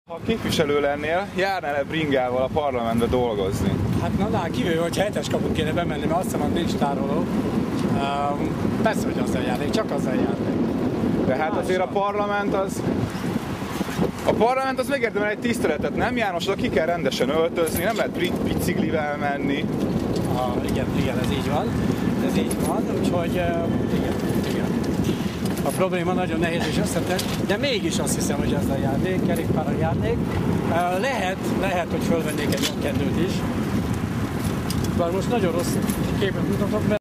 László János bevallja!
Igen! Járna biciklivel a Parlamentbe, ahogy azt a másik videón is teszi. már akinek nem tűnt föl...